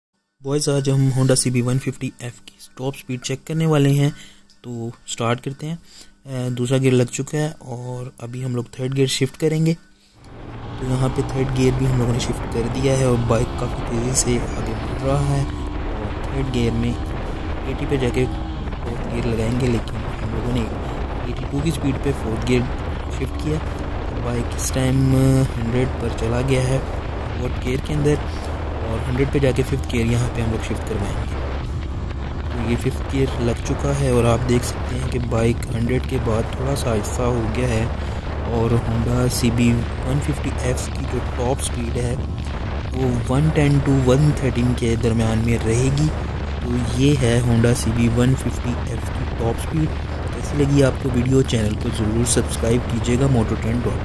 top speed honda cb150f 2023 model
In this video you can see bike top speed
check bike torque
o to 100kmph